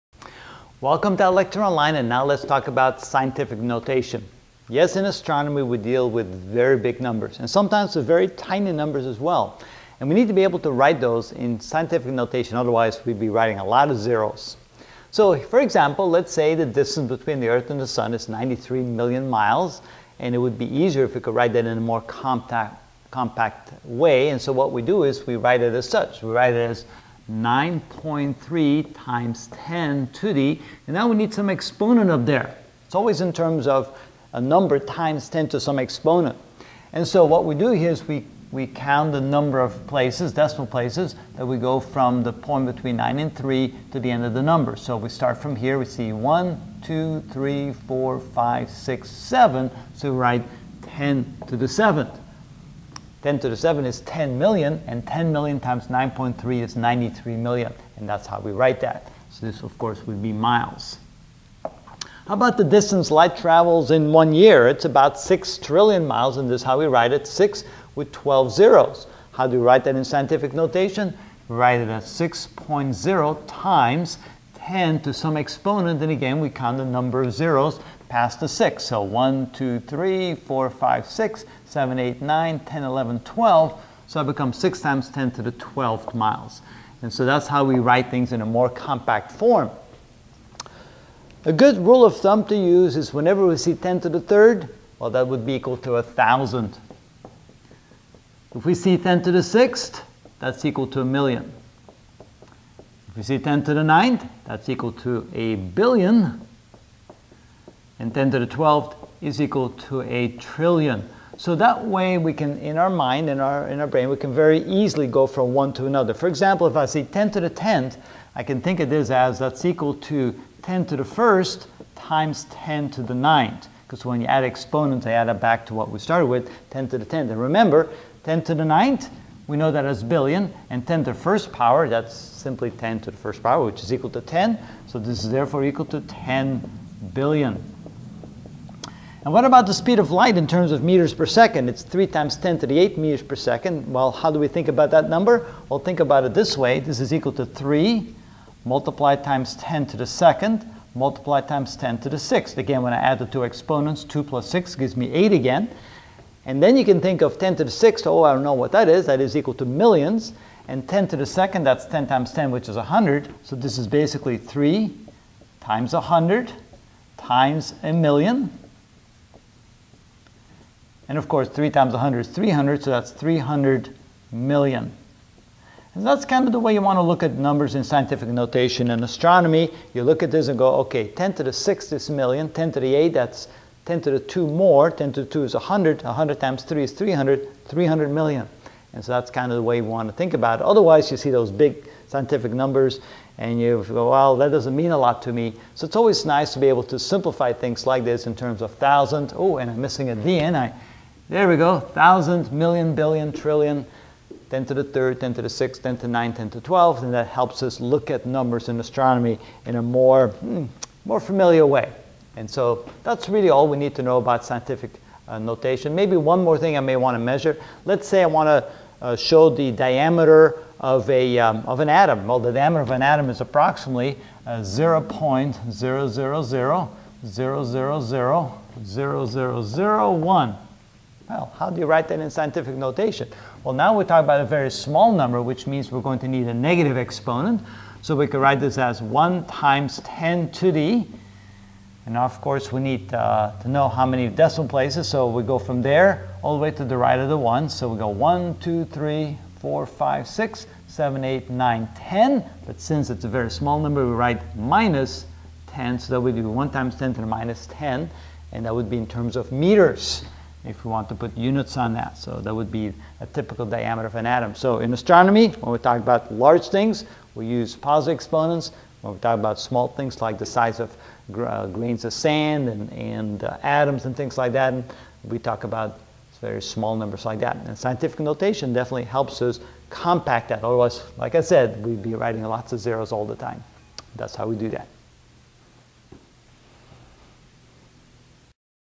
Astronomy - Chapter 1: Introduction (9 of 10) How to Use Scientific Notation in Astronomy
In this video I will discuss how scientific notation is used in astronomy.